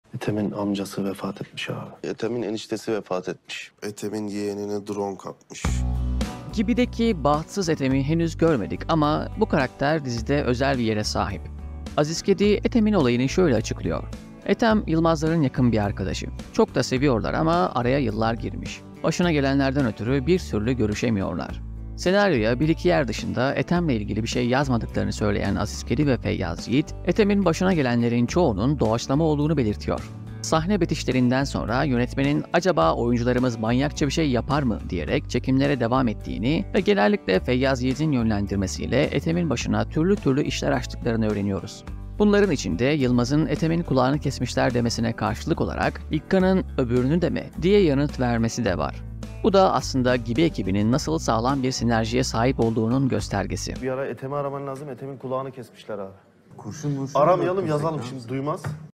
Ethem'in Başına Gelenler Doğaçlamaymış! - GİBİ
Feyyaz Yiğit ve Aziz Kedi, Gibi dizisinde görünmemesine rağmen başına gelenlerden ötürü dikkat çeken bir karakter olan Ethem'in olayını anlatıyor.

Feyyaz Yiğit, Gibi, Aziz Kedi, Exxen, İlkkan, Yılmaz, Ersoy, Ethem, Komedi, Yerli Dizi